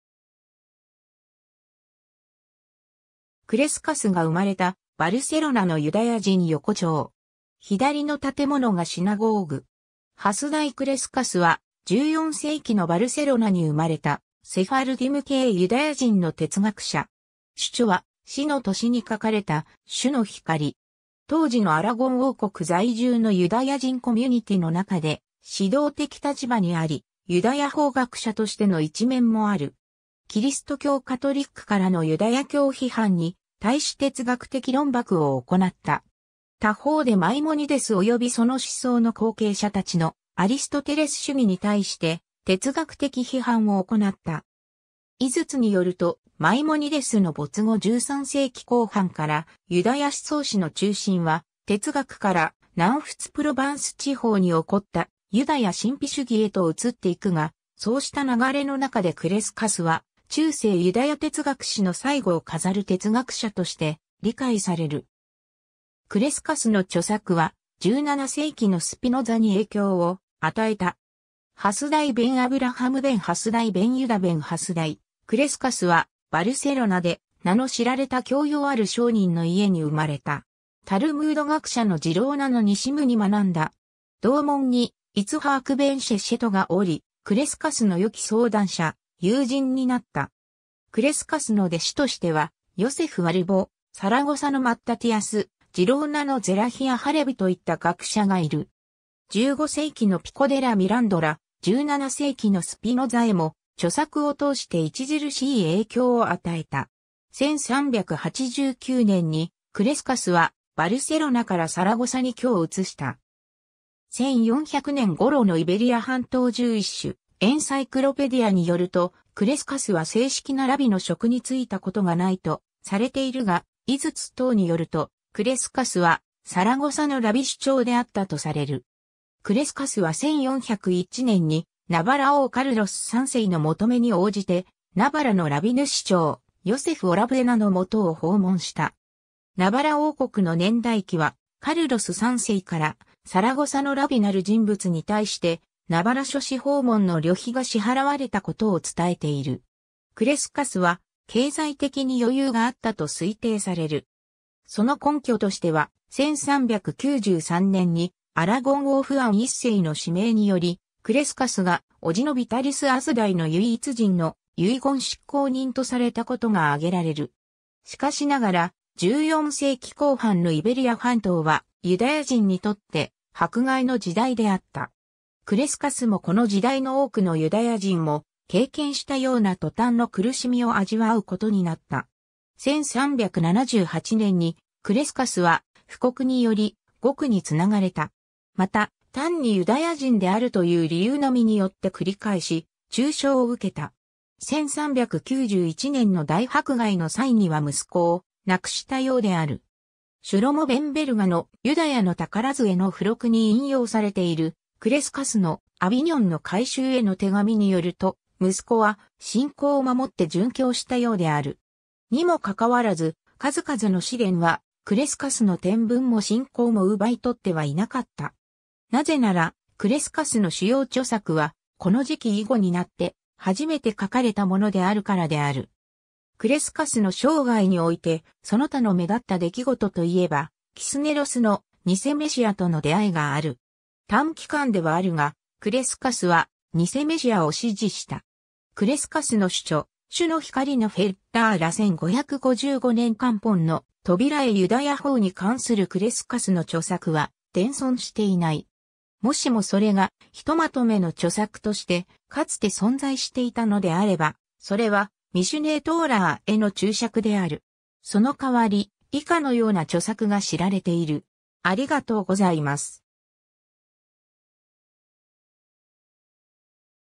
ハスダイ・クレスカス
クレスカスが生まれたバルセロナのユダヤ人横丁。
左の建物がシナゴーグ。
 ハスダイ・クレスカス（ヘブライ語: חסדאי קרשקש‎, Ḥasdai Crescas, 1340年頃 - 1410年頃）は、14世紀のバルセロナに生まれたセファルディム系ユダヤ人の哲学者。
主著は死の年に書かれた『主の光』。
当時のアラゴン王国在住のユダヤ人コミュニティの中で指導的立場にあり、ユダヤ法学者としての一面もある。
キリスト教カトリックからのユダヤ教批判に対し哲学的論駁を行った。
他方でマイモニデス及びその思想の後継者たちのアリストテレス主義に対して哲学的批判を行った。
井筒 (1988)によると、マイモニデスの没後13世紀後半から、ユダヤ思想史の中心は哲学から、南仏プロヴァンス地方に興ったユダヤ神秘主義（カバラ）へと移っていくが、そうした流れの中でクレスカスは、中世ユダヤ哲学史の最後を飾る哲学者として理解される。
クレスカスの著作は、17世紀のスピノザに影響を与えた。
 ハスダイ・ベン・アブラハム・ベン・ハスダイ・ベン・ユダ・ベン・ハスダイ・クレスカス（Hasdaï ben Abraham ben Hasdaï ben Juda ben Hasdaï Crescas）は、バルセロナで名の知られた教養ある商人の家に生まれた。
タルムード学者のジローナのニシム（RaN）に学んだ。
同門にイツハーク・ベン・シェシェト（RIBaSH）がおり、クレスカスのよき相談者、友人になった。
クレスカスの弟子としては、ヨセフ・アルボ、サラゴサのマッタティアス（He-Ḥaluẓ）、ジローナのゼラヒア・ハレヴィといった学者がいる。
15世紀のピコ・デラ・ミランドラ、17世紀のスピノザへも著作を通して著しい影響を与えた。
 1389年にクレスカスはバルセロナからサラゴサに居を移した。
 1400年ごろのイベリア半島 Jewish Encyclopedia (1906) によると、クレスカスは正式なラビの職に就いたことがないとされているが、井筒 (1988) 等によるとクレスカスは「サラゴサのラビ主長」であったとされる。
クレスカスは1401年に、ナバラ王カルロス3世の求めに応じて、ナバラのラビ主長、ヨセフ・オラブエナ（Juze (Joseph) Orabuena）のもとを訪問した。
ナバラ王国の年代記は、カルロス3世から「サラゴサのラビ」なる人物に対してナバラ諸市訪問の旅費が支払われたことを伝えている。
 クレスカスは経済的に余裕があったと推定される。
その根拠としては、1393年にアラゴン王フアン1世の指名によりクレスカスがおじのビタリス・アズダイ（Vitalis Azday）の唯一人の遺言執行人とされたことが挙げられる。
しかしながら、14世紀後半のイベリア半島はユダヤ人にとって迫害の時代であった。
クレスカスもこの時代の多くのユダヤ人も経験したような塗炭の苦しみを味わうことになった。
1378年にクレスカスは誣告により獄に繋がれた。
また、単にユダヤ人であるという理由のみによって繰り返し中傷を受けた。
1391年の大迫害（6月のセビージャにおける反ユダヤ暴動に端を発した迫害事件）の際には息子を亡くしたようである。
シュロモ・ベン・ヴェルガの『ユダヤの宝杖』（Shevet Yehuda）の付録に引用されているクレスカスの 『アヴィニョンの会衆への手紙』によると、息子は信仰を守って殉教したようである。
 にもかかわらず、数々の試練はクレスカスの天分も信仰も奪い取ってはいなかった。
なぜならクレスカスの主要著作はこの時期以後になってはじめて書かれたものであるからである。
クレスカスの生涯においてその他の目立った出来事といえば、キスネロスの偽メシアとの出会いがある。
短期間ではあるが、クレスカスは偽メシアを支持した。
 クレスカスの主著、『主の光』（Or Adonai, אור אֲדֹנָי‎）のフェッラーラ1555年刊本の扉絵 ユダヤ法（ハラーハー）に関するクレスカスの著作は伝存していない。
もしもそれが一まとめの著作として、かつて存在していたのであれば、それは『ミシュネー・トーラー』への注釈である。
その代わり、以下のような著作が知られている。